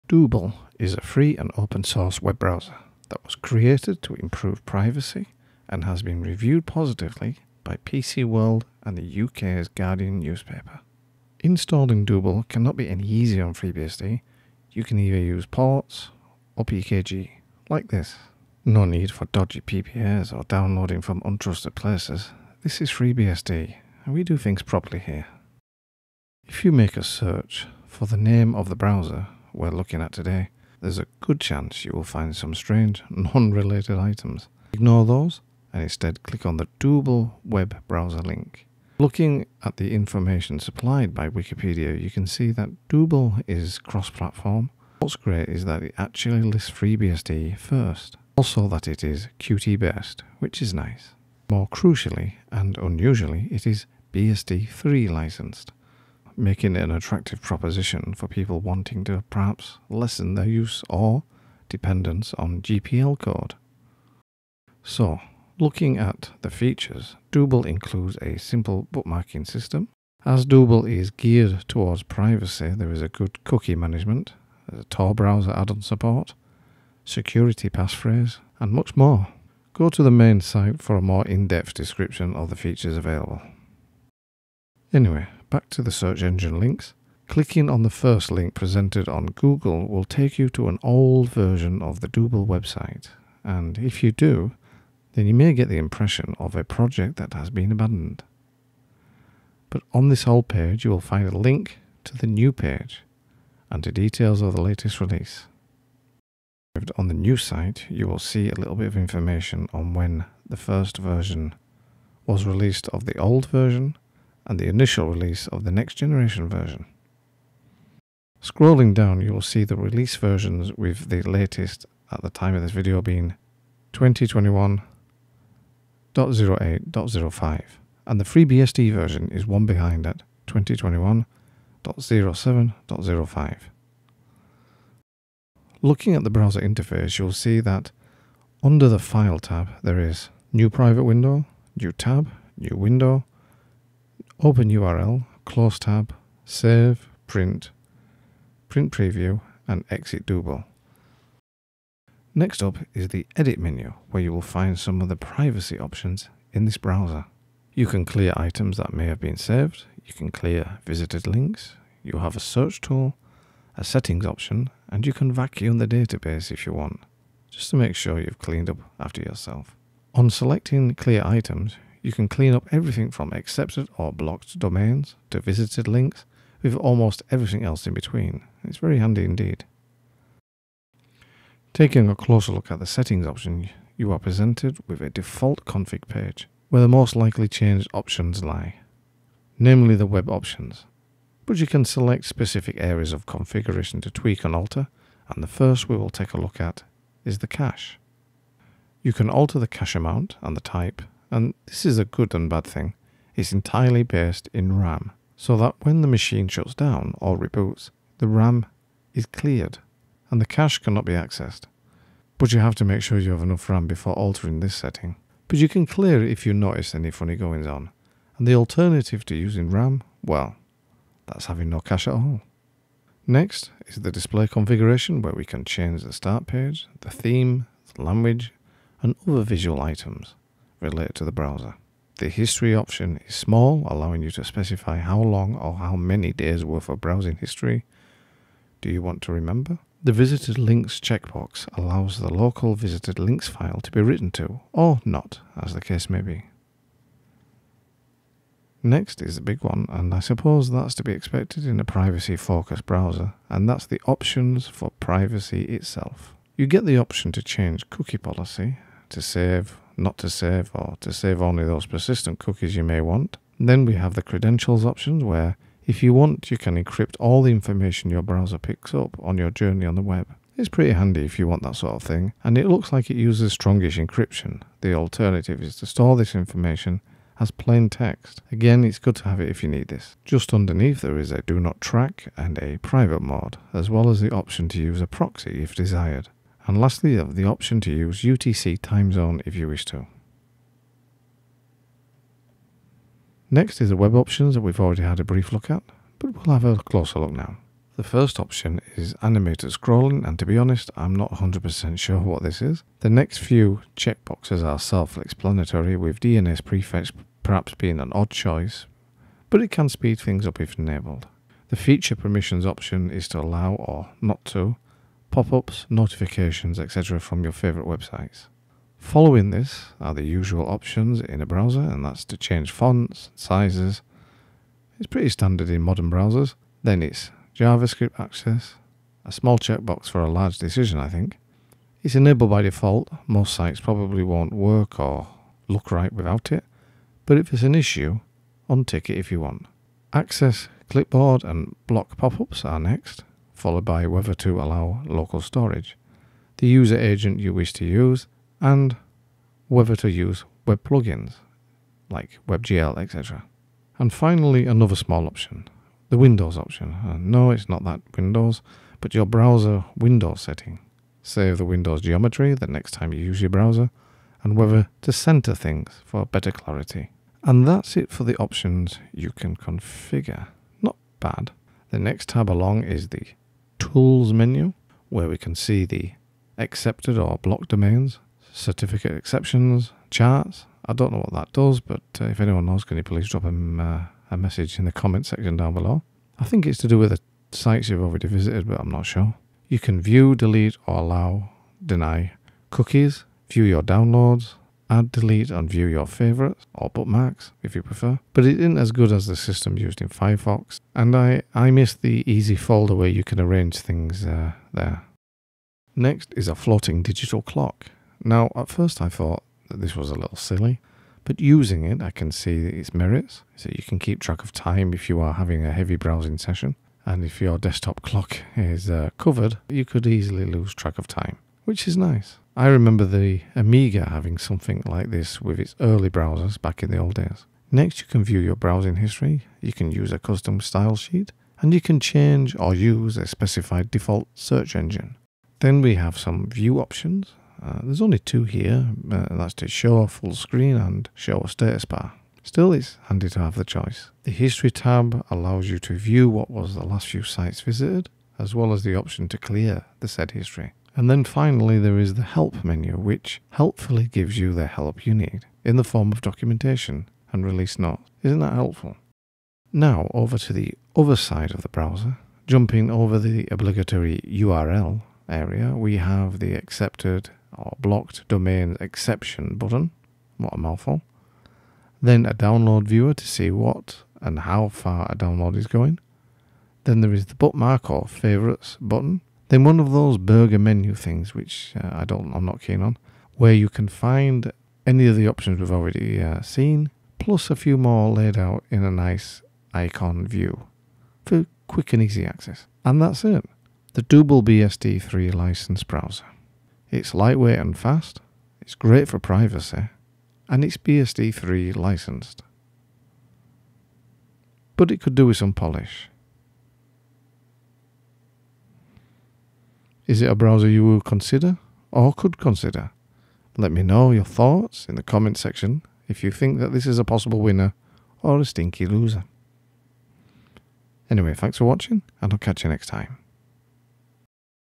A Dooblecious BSD-3 Browser!
Something different, here is a quick look at a lightweight and security focused browser that may appeal to jaded users of Firefox & Chrome.